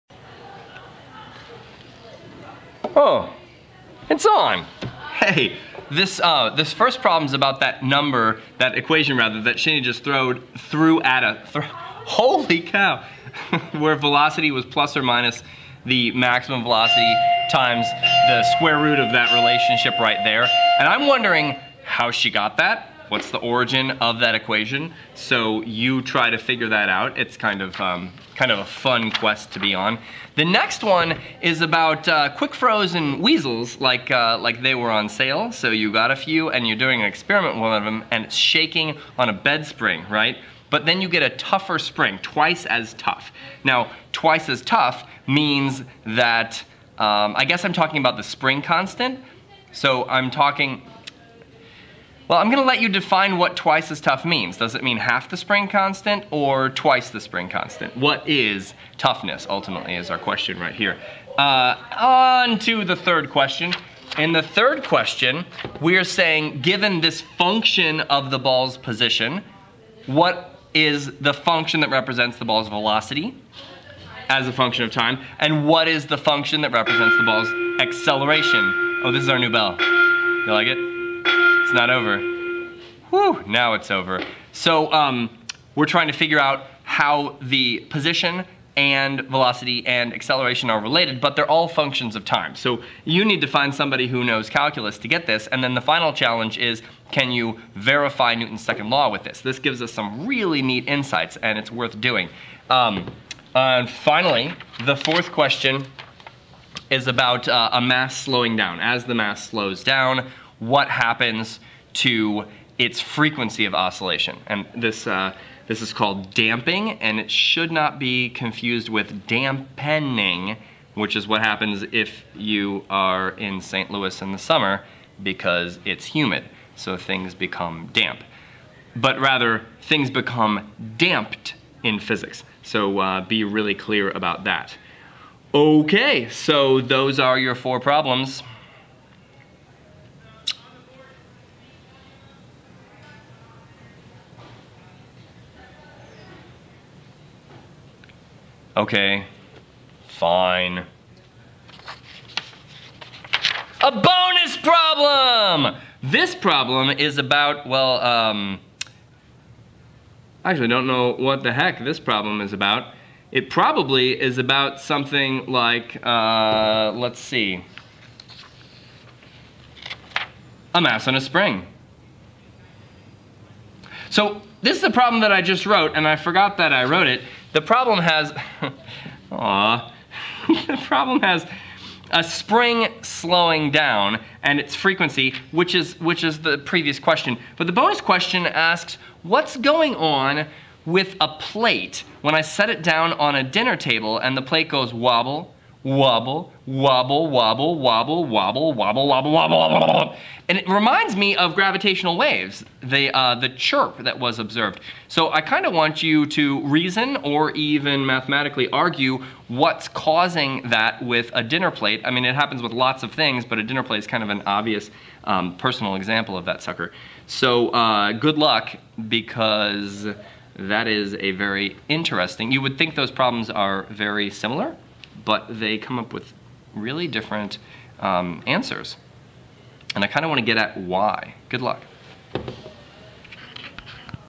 Simple Harmonic Motion Problemz | CCP16 | 'Sup Crash Course | Doc Physics
Here I have provided a bunch of supplemental problems to enrich a person's self-study of physics through Crash Course's slammin' physics series.  Remember always that you gain understanding of physics by DOING it, regardless of how good your teacher is.

Oh, it works like this:  once you've seen the problems, you should pause this video and work them.

Care to watch a (slightly) slower and deeper approach to the material?  Here is a link to my playlist on simple harmonic motion:

If you've got some answers, POST THEM IN THE COMMENTS.  I will neither confirm or deny your work, so feel free to discuss unhampered by "authority."  If interest in this series picks up by a factor of ten, I will make video answers.  Get 'em, tiger!